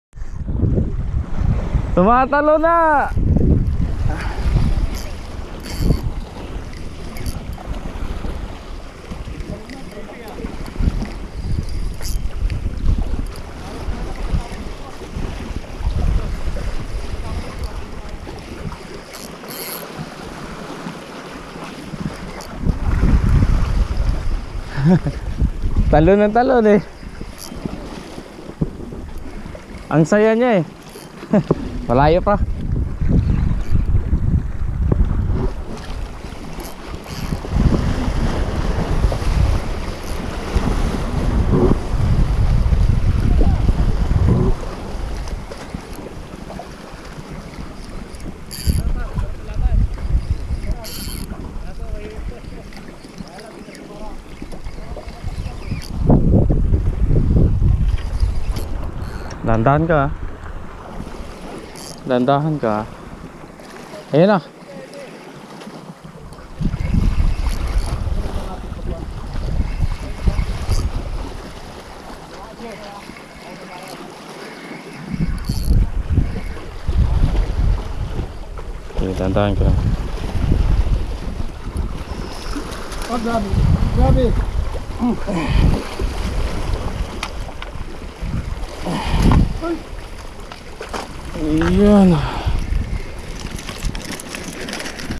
QueenFish by Balolong
Reel: Shimano Symetre 4000FL
Rod:  Kashima Arkol 2.7m
Line: G-soul X8 Upgrade
Lure: Megabass Kanata Gold